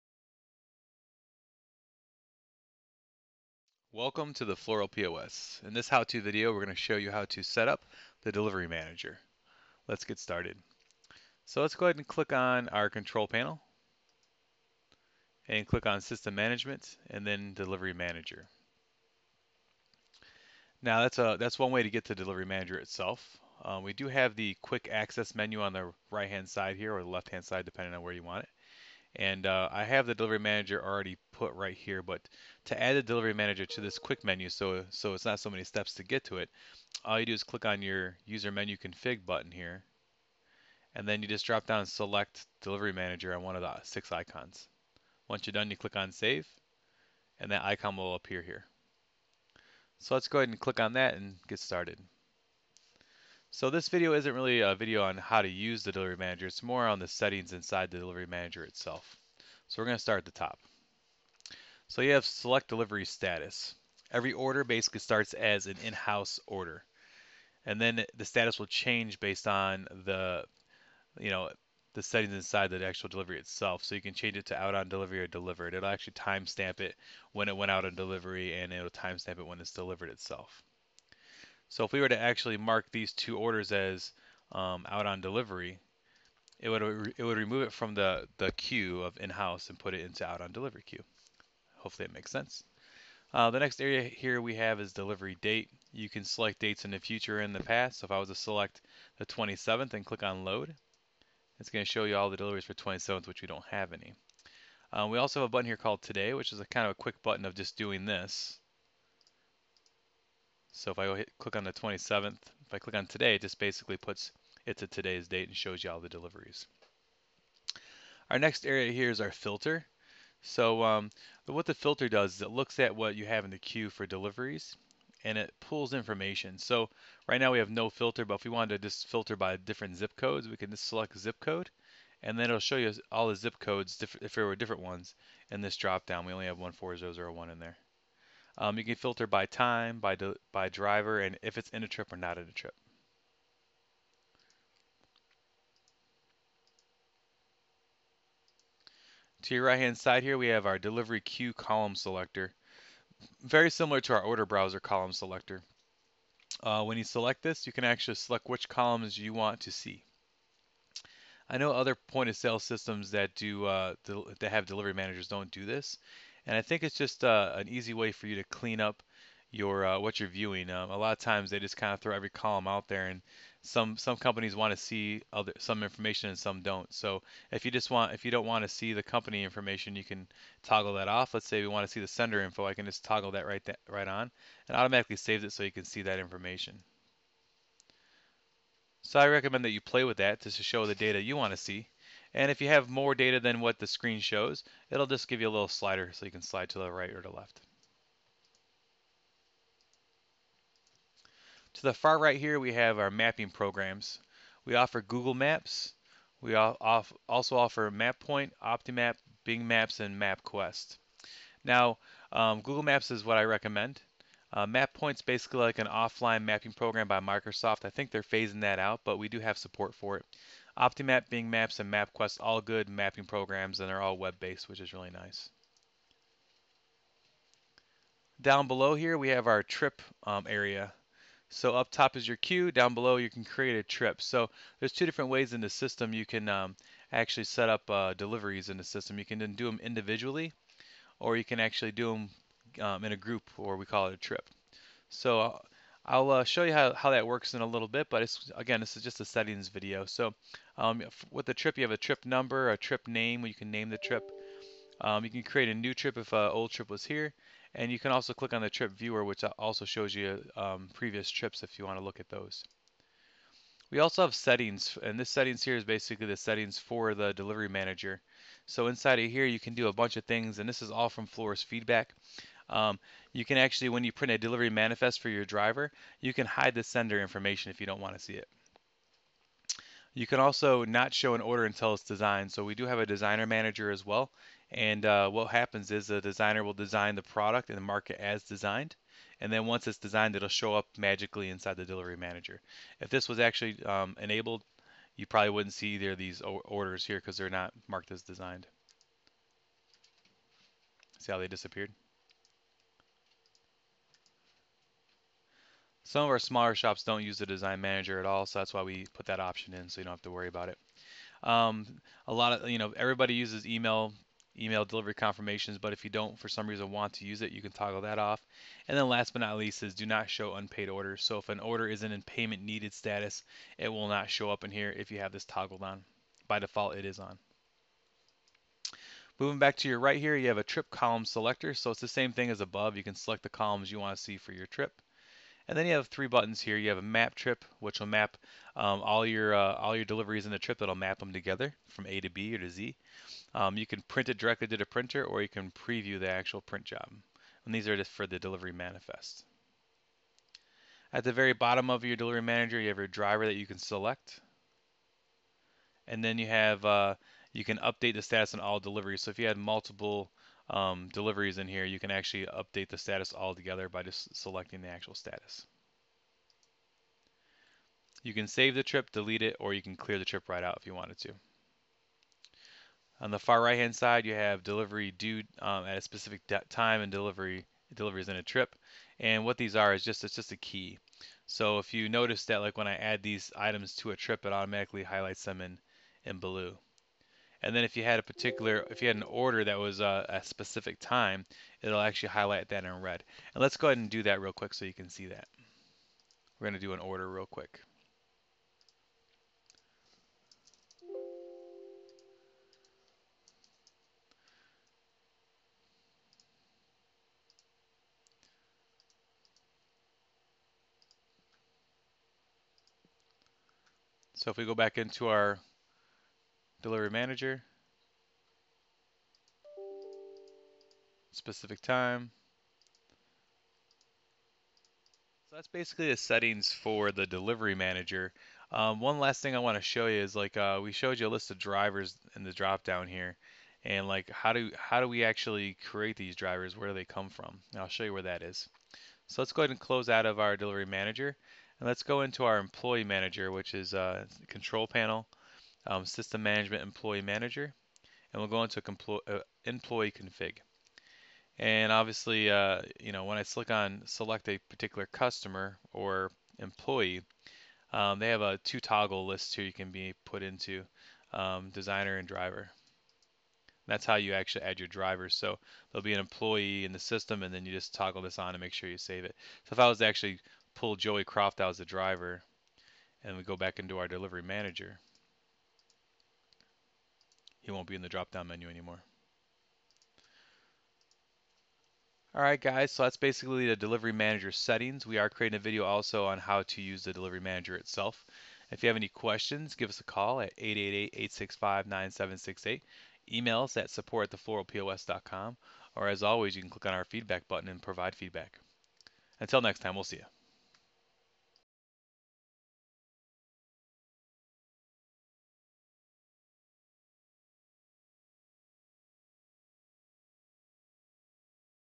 HOW TO - DELIVERY MANAGER SETTINGS
We show you how to setup the Delivery Manager and the settings within. Enjoy.